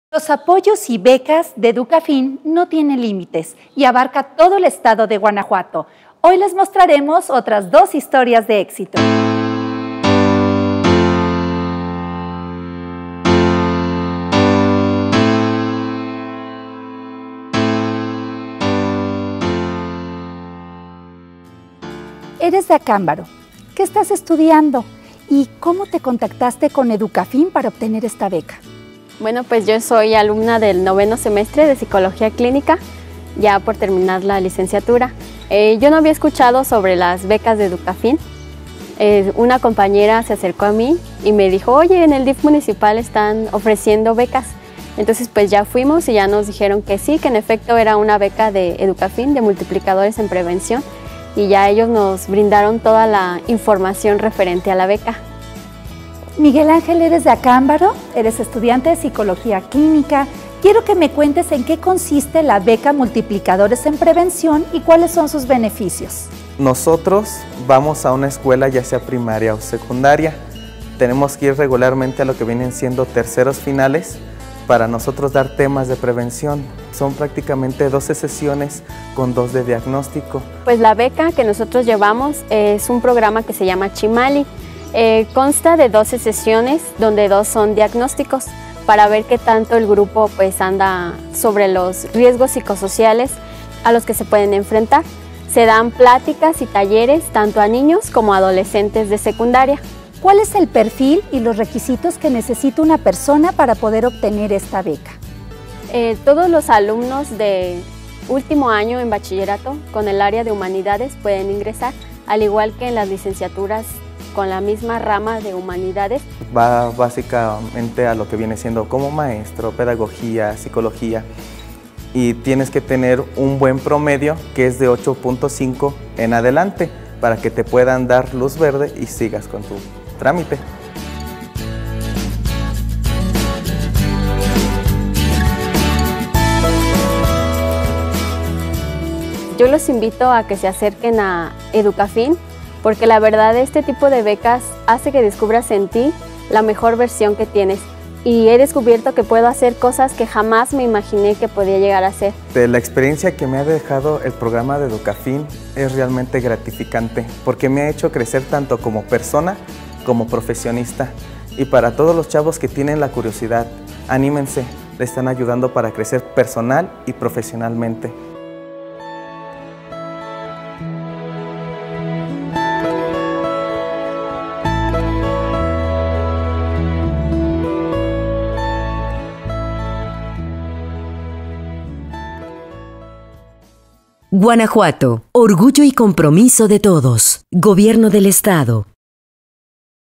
Lizbeth y Miguel Ángel, Programa Multiplicadores en Prevención EDUCAFIN
Este Programa, ha logrado reunir a varios jóvenes con gran preocupación por el bienestar social. Formando grupos de apoyo donde se les aconseja y guía. Lizbeth y Miguel Ángel son parte de este Programa brindando su apoyo a niños de Educación Primaria y Secundaria.

Imagenes: Alejandro Luna y Juan José Plascencia
Edición y realización: Juan José Plascencia